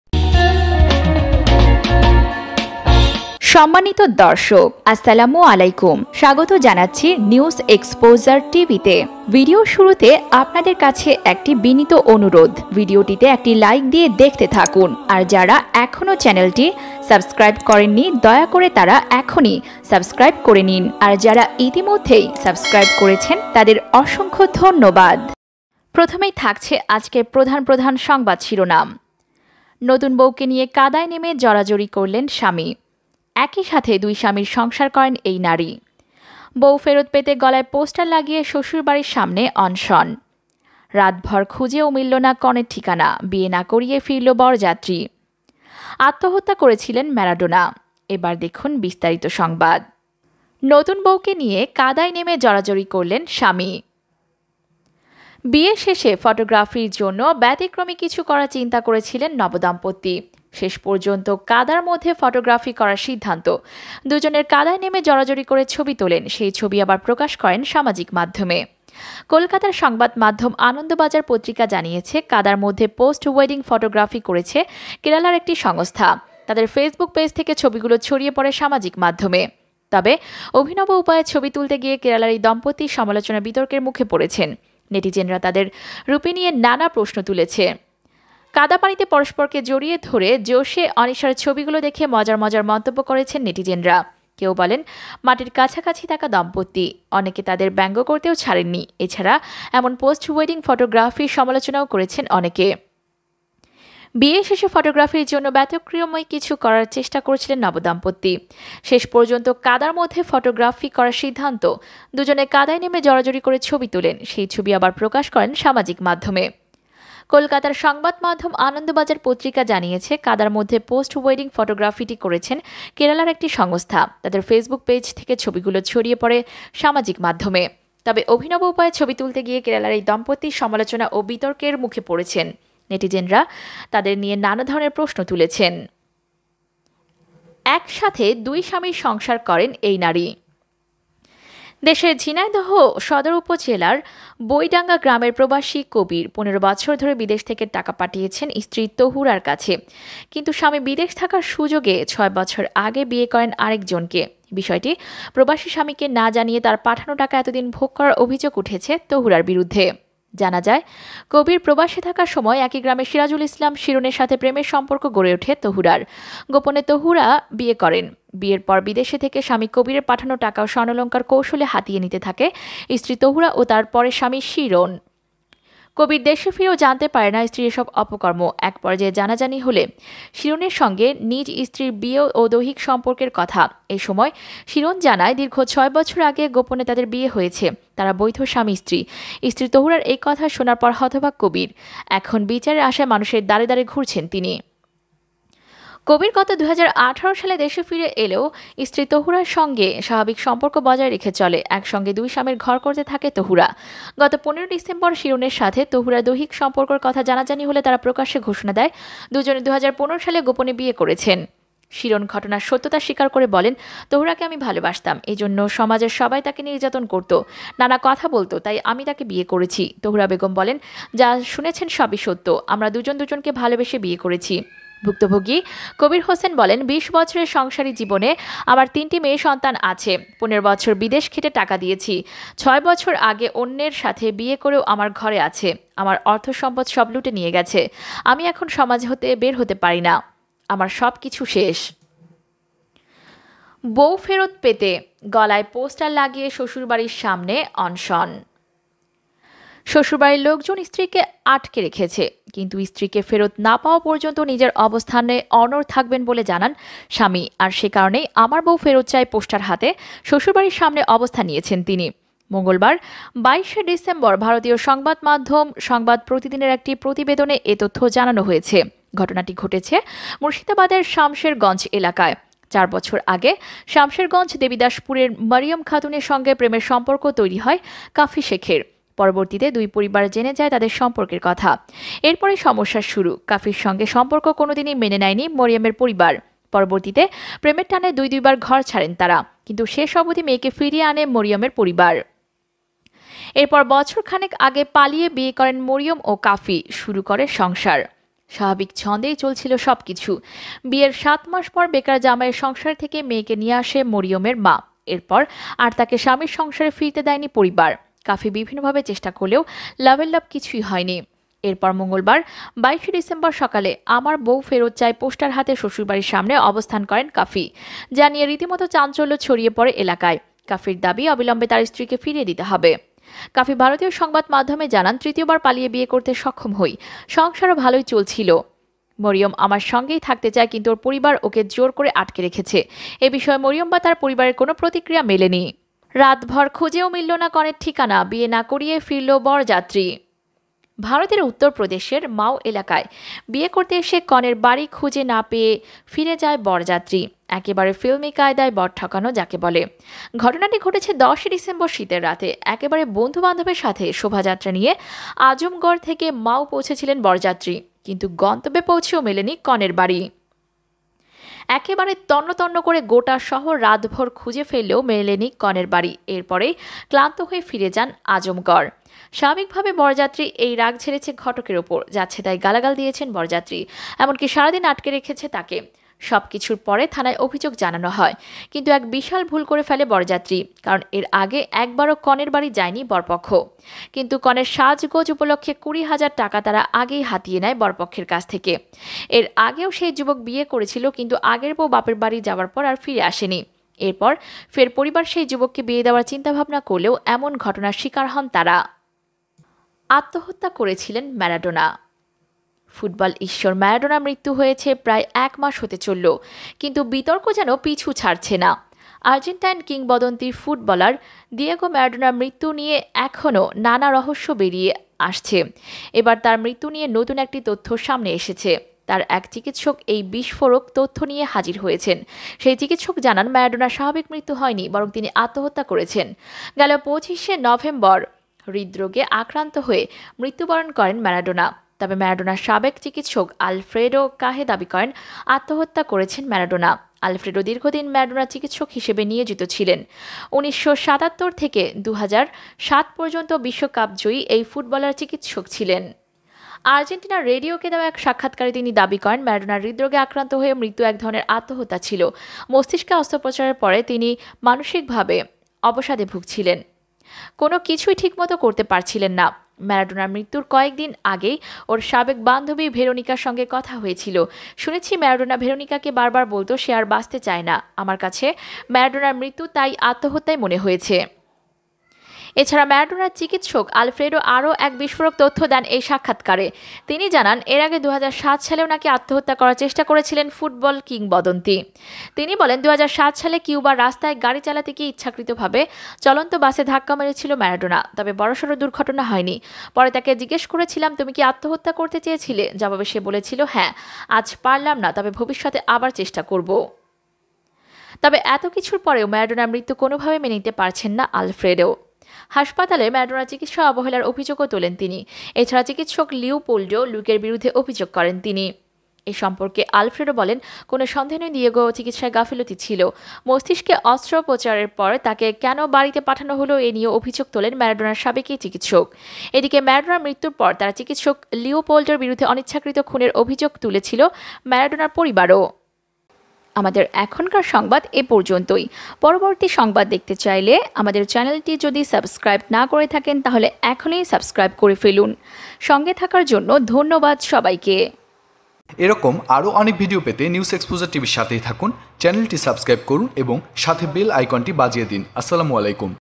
যে কারণে নব দম্পত্তিকে কাদায় চুবানো হলো
যে কারণে নতুন বউকে নিয়ে কাদায় নেমে মাখামাখি খেললো আতপাগল নব দম্পত্তি